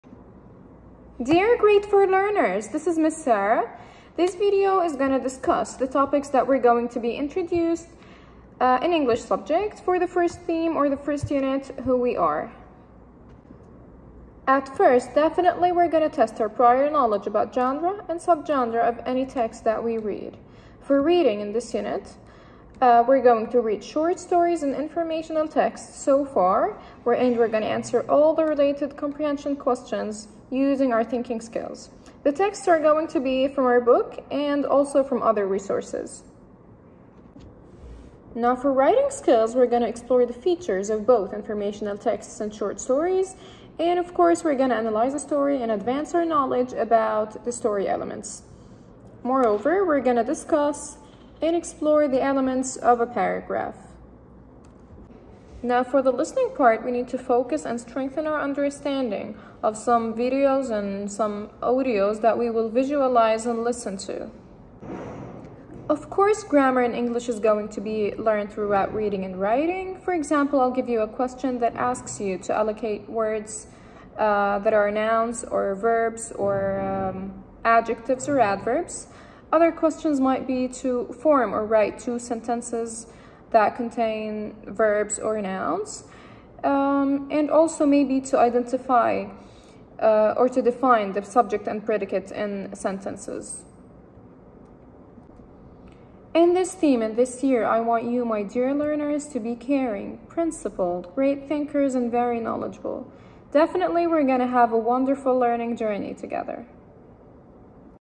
English - Week 2 - Grade 4 - By Ms. Sarah AlShalabi
English Week 2 Description

This video is a brief overview of the what learners are going to be studying in English in the 2nd week of school. In English subject, learners are going to be introduced to informational texts and a short story to analyze and solve related comprehension reading questions. Moreover, learners will study elements of a paragraph and grammar lessons that discuss nouns, verbs, subjects and predicates.